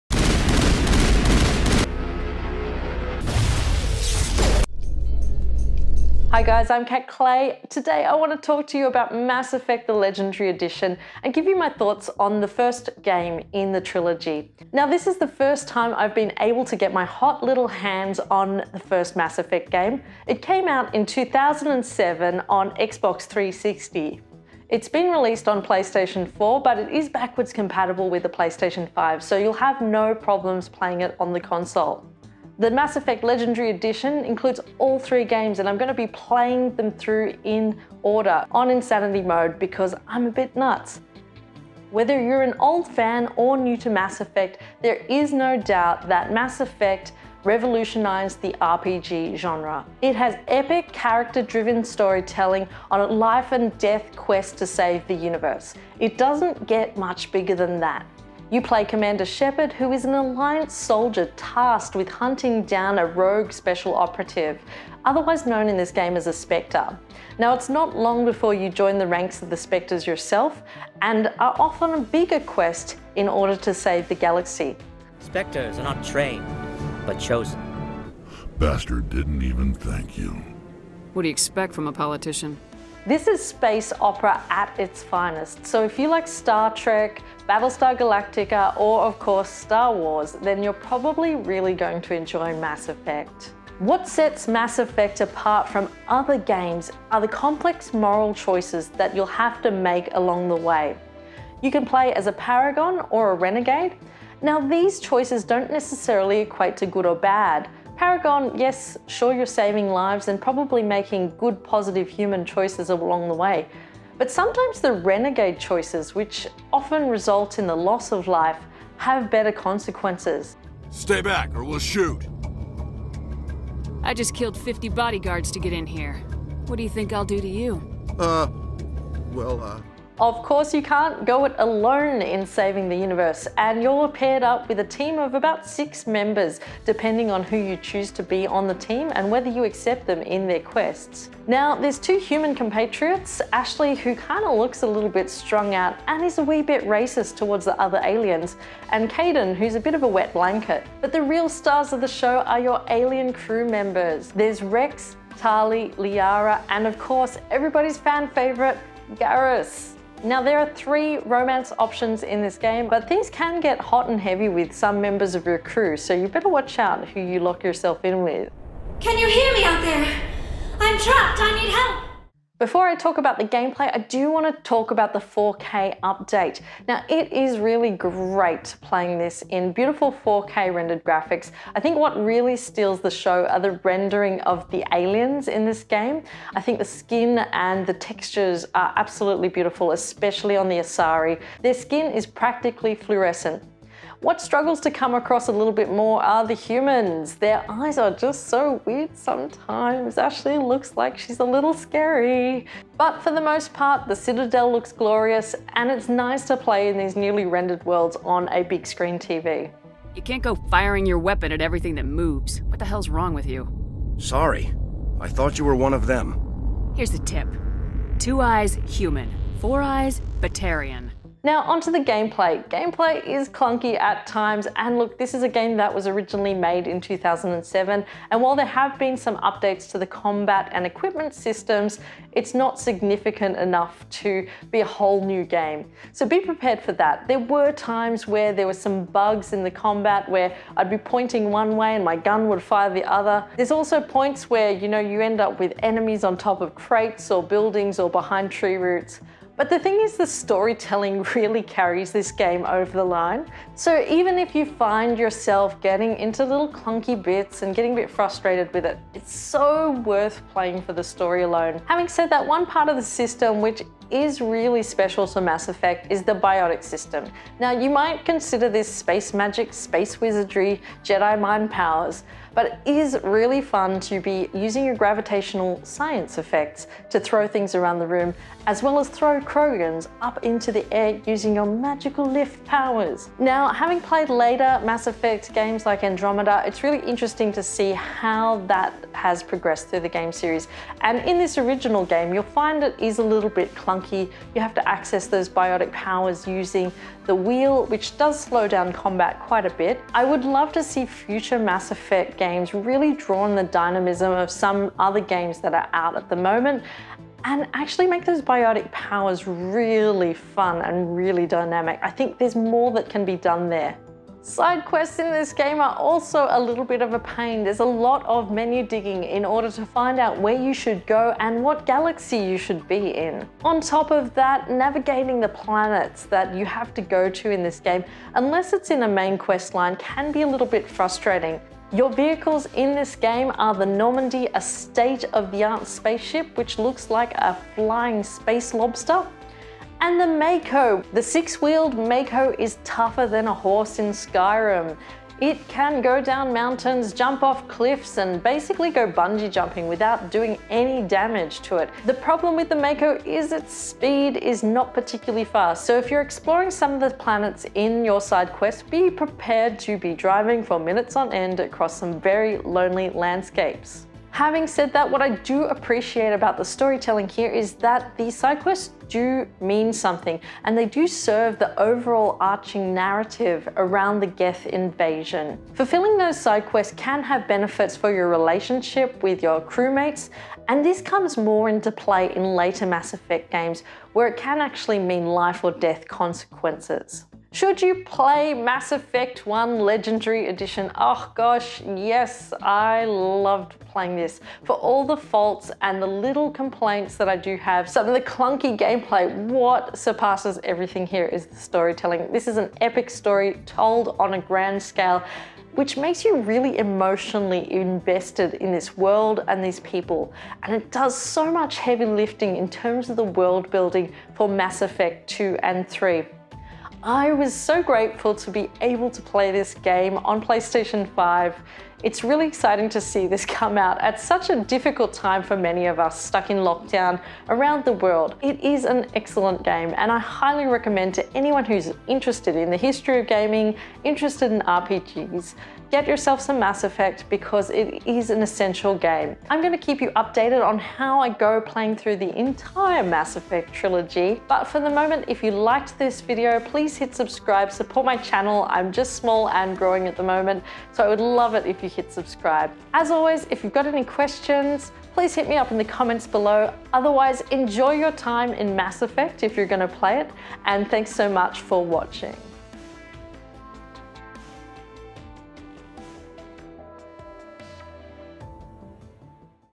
Mass Effect: Legendary Edition PS5 Review
If you're new to Mass Effect or an old fan, there's no denying the Mass Effect trilogy revolutionised the RPG genre. So it's brilliant that we can now play it in sweet, sweet 4K on PS5. I just finished Mass Effect 1 in the Legendary Edition on insanity mode 🤪, so here are my thoughts!